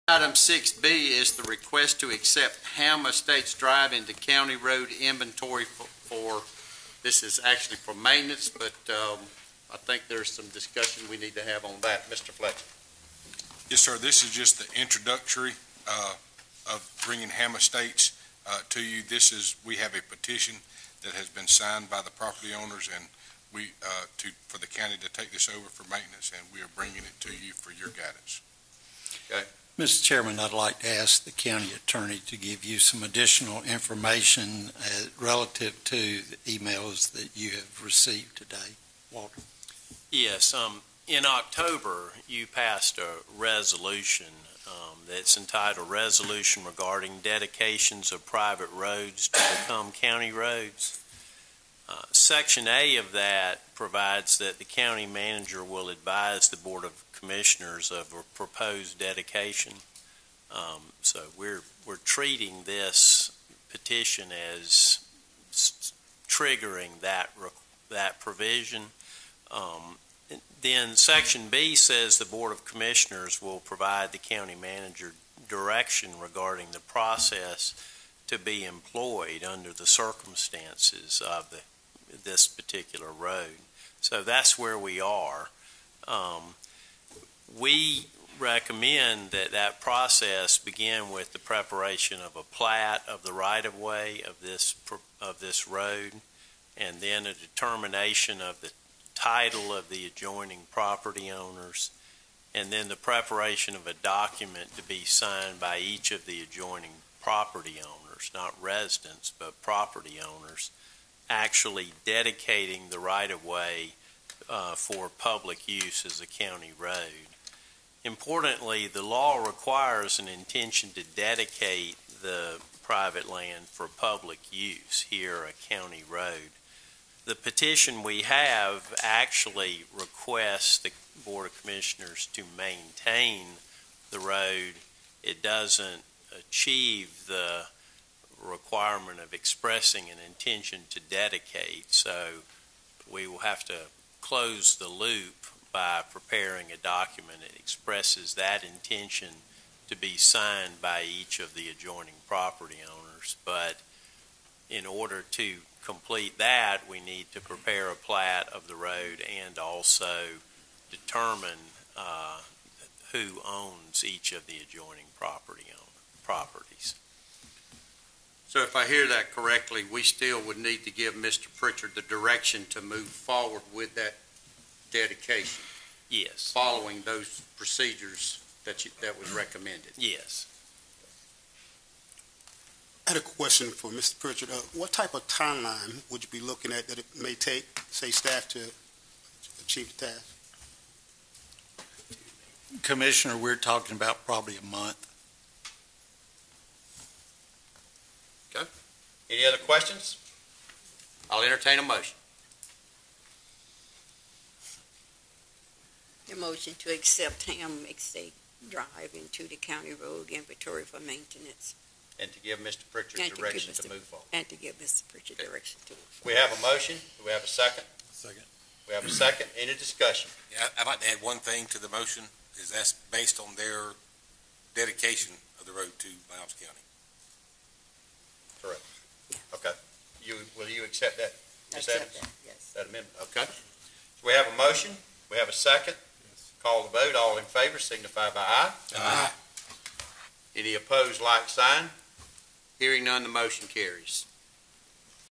6b. Accept Hamm Estates Dr. into the County Road Inventory @ LCC Regular - 2020-02-25
6b. Request to Accept Hamm Estates Dr. into the County Road Inventory for Maintenance